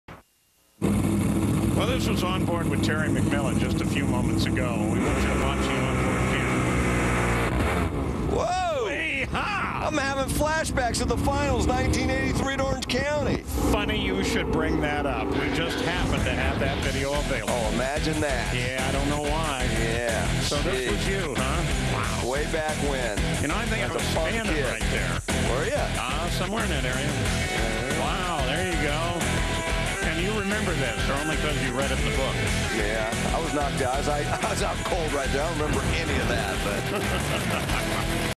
Mike Dunn Has Orange County Finals 1983 Flashbacks Fire Crash Wreck.mpg
Mike Dunn Has Orange County Finals 1983 Flashbacks Fire Crash Wreck